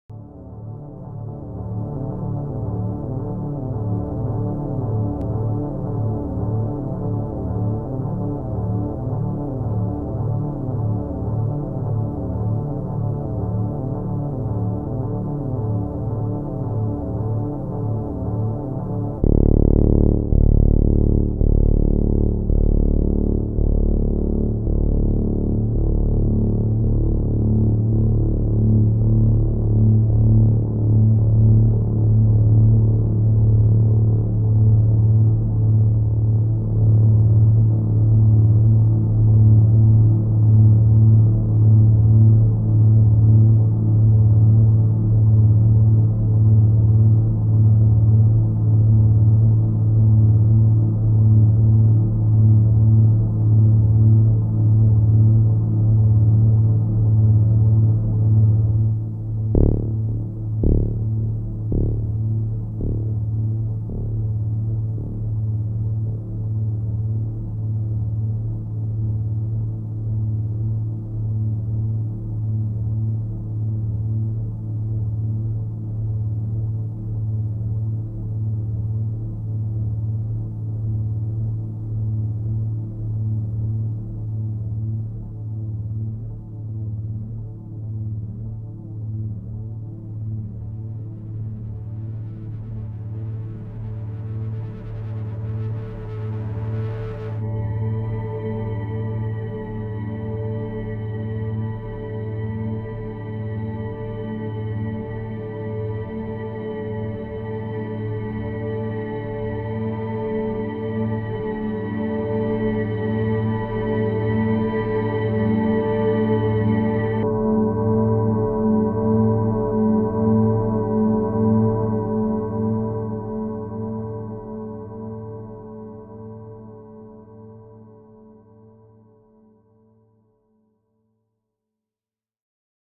Nebula of red dust and blue stars
NGC 2264 which is approximately 2500 light years away, has blue stars surrounded by red dust. The sounds were produced using an ocean machine and a JEN SX1000 synth.